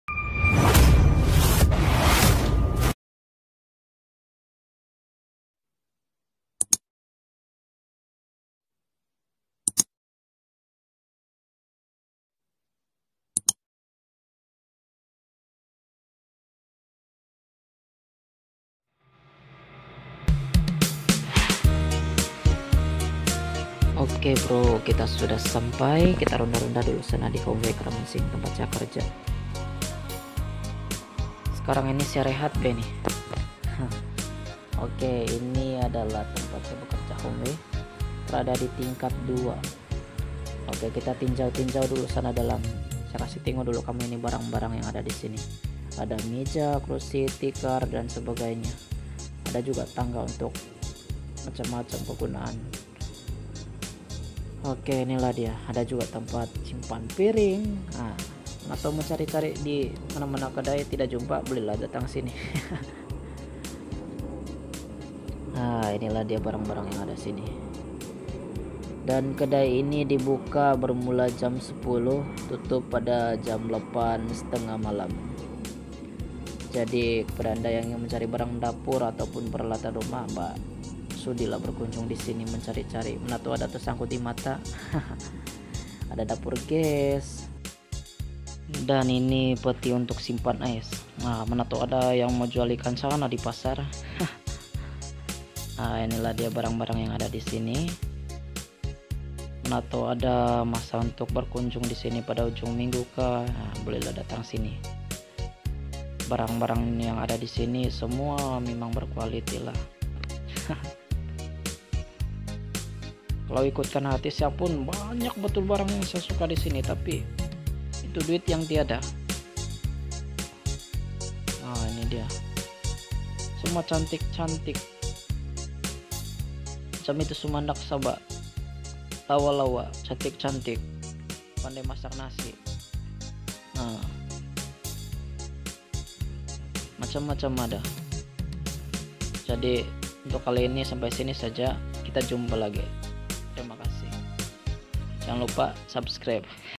1 Homeware Karamunsing
Jom Singgah membeli-belah
Di Homeware..
Barang dapur/Rumah ada disini.
Tingkat 2